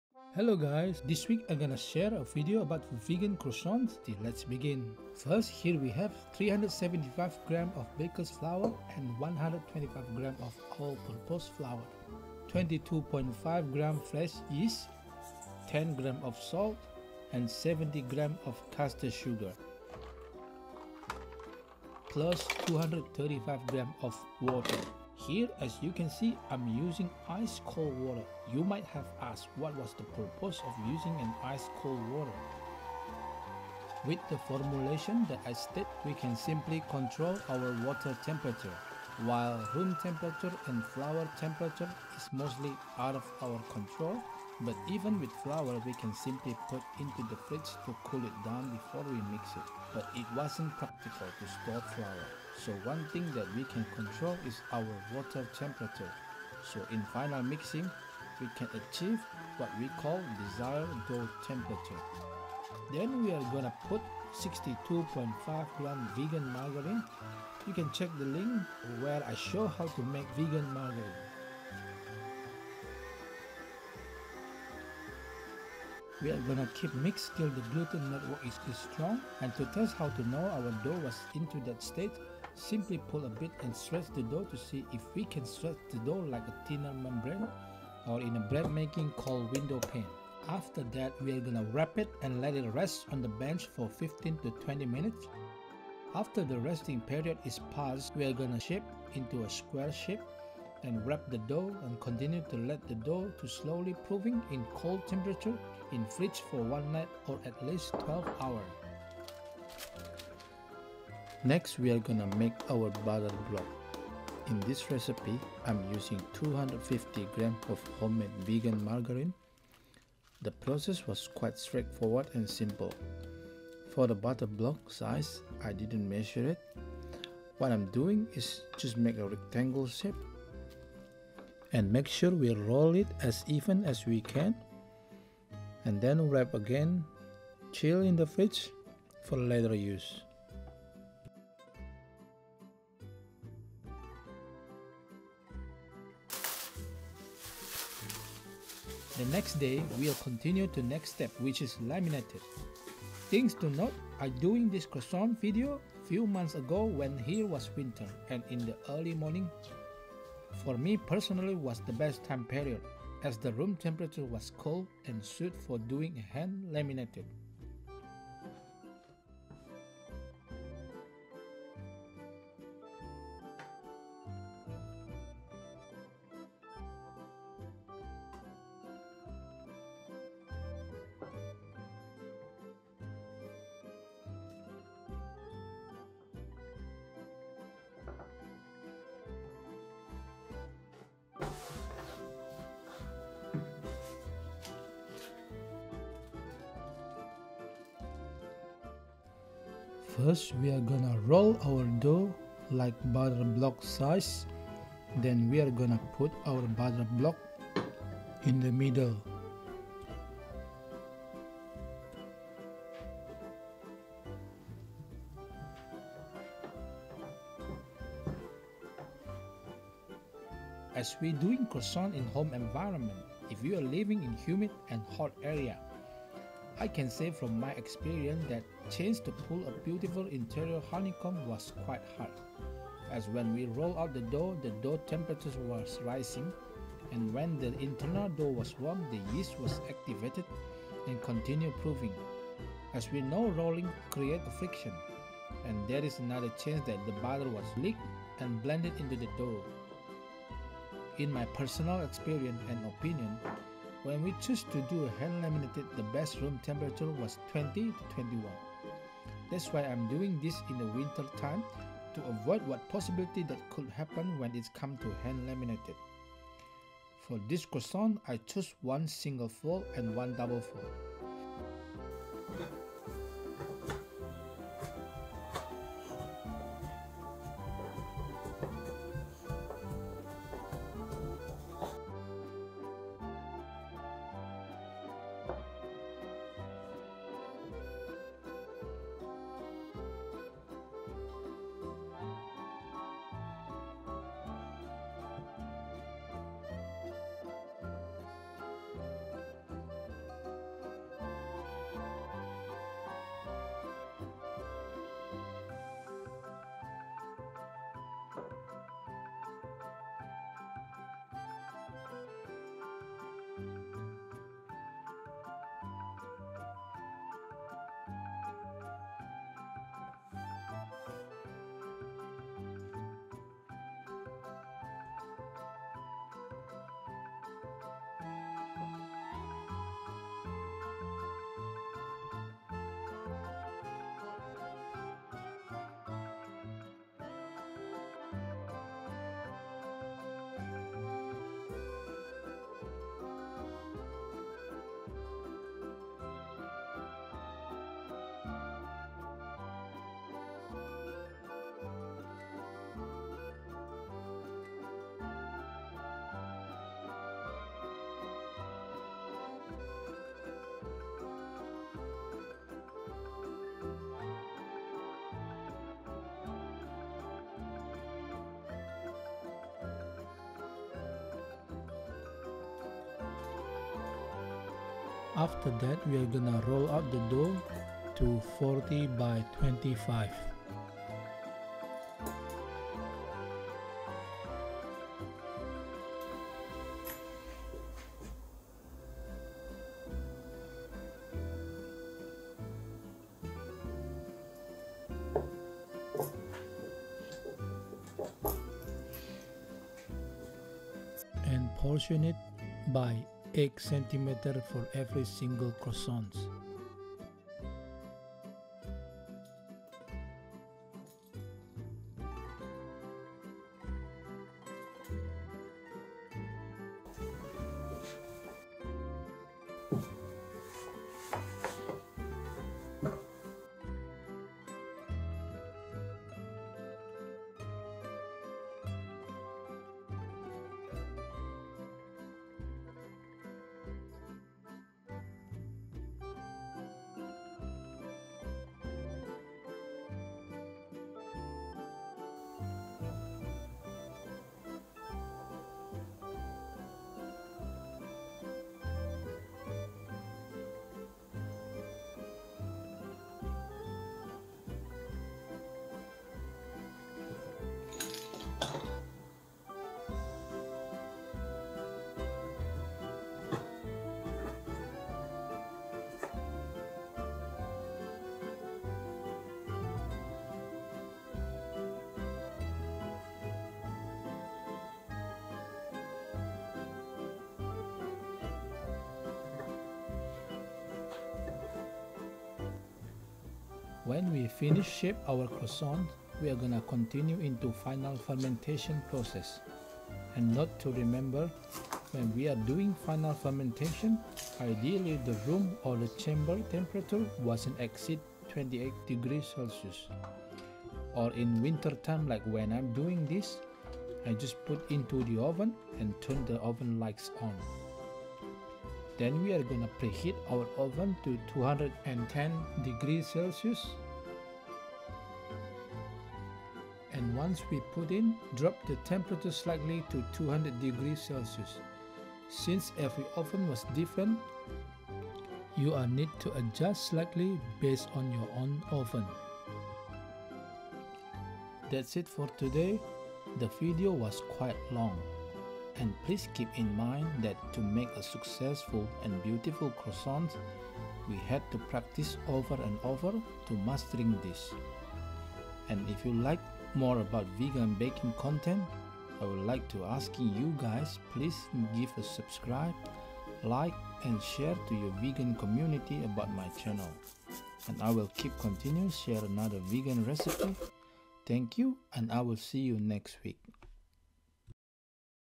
How to make a vegan croissant [純素牛角包】
hello guys

this week i share a video about how to make your own vegan croissant from scratch

making a croissant was never an easy task, since it was involved so many step and technique, we need to practice over and over to had a better understanding about making croissant

but it wasn't an impossible task, as long we know how to make our croissant dough, laminating process correctly, i think everyone can pull out a beautiful croissant bread at home style environment

Thank You and i hope you enjoy the video